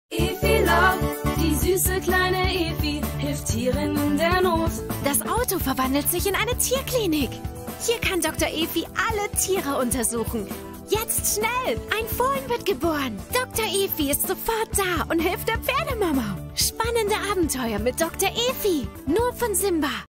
Đồ Chơi Búp Bê Bác Sĩ EVI LOVE Doctor Evi 2-in-1 Animal
Búp Bê Bác Sĩ EL Doctor Evi 2-in-1 Animal Mobile không chỉ là một món đồ chơi thông thường mà còn là người bạn nhỏ đáng yêu của các bé gái.
Bộ đồ chơi bao gồm 1 cô bé Evi, 2 bạn nhỏ thú cưng, 1 chiếc xe cứu thương và nhiều dụng cụ chăm sóc thú cưng. Các bé sẽ được nhập vai vào những bác sĩ nhí, chăm sóc bạn nhỏ thú cưng đáng thương.

👉 Đồ chơi thuộc thương hiệu Evi Love - trực thuộc tập đoàn Simba Dickie Group đến từ Đức.